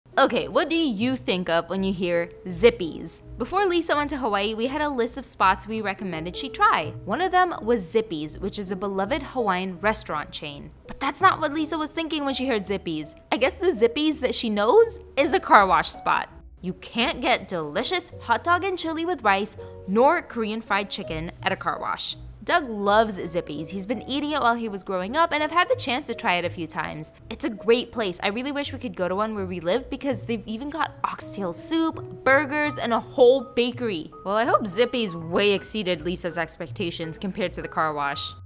Lisa thought this was a car wash
There’s a ton of Zippy’s locations throughout Hawaii! Eating with @LisaNguyen